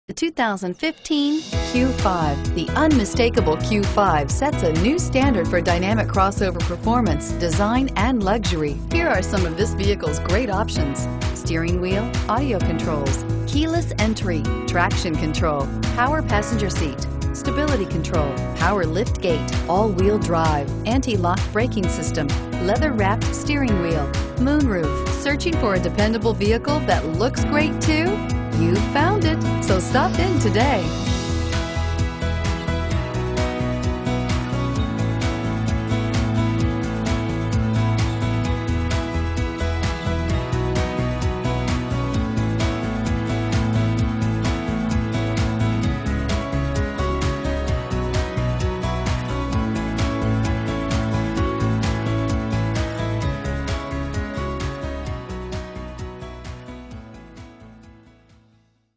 2015 Audi Q5 FREEHOLD NJ 150976X
2015 Audi Q5 Premium Plus

Ray Catena Audi of Freehold
3561 US Route 9

Looking for the right Sport Utility? Today could be your lucky day. With 0 miles, this Moonlight Blue Metallic, 2015 Audi Q5 equipped with Automatic transmission could be yours. Request more information and set up a test drive right away.